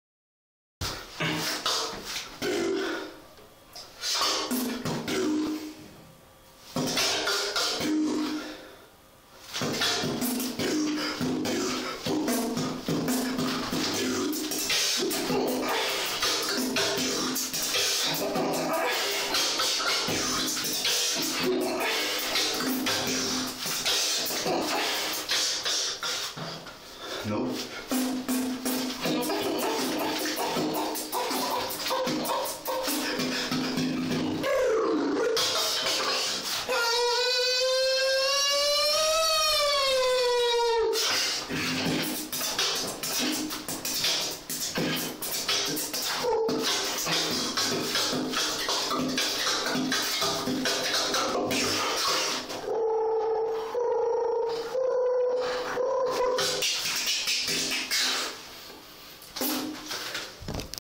Re;Lytos beatbox
I am a Japanese beatboxer. I am 18 years old.

I really like Lytos,I respect very much.